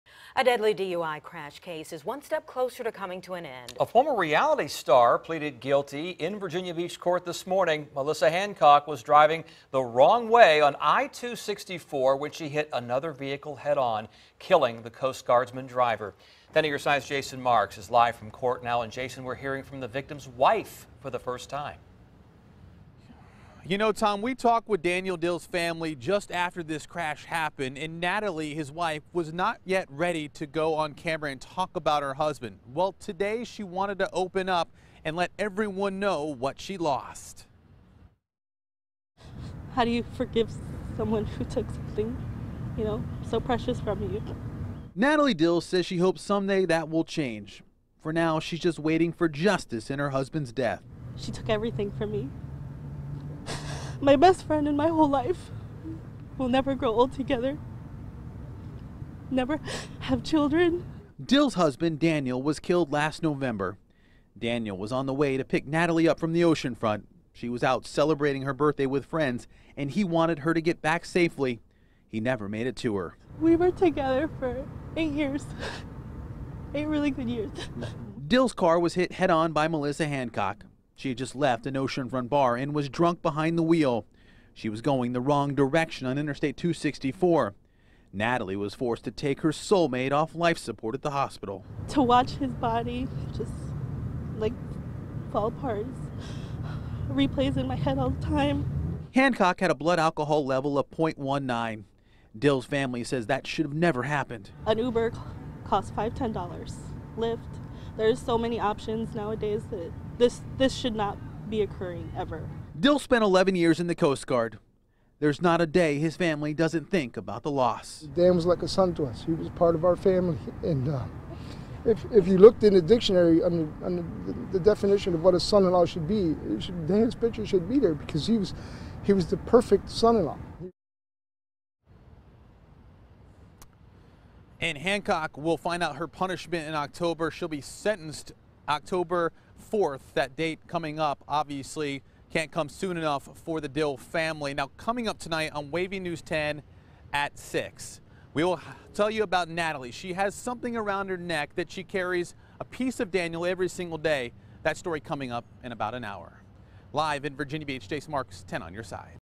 Former reality TV star pleads guilty in deadly DUI crash
Jason Marks reports.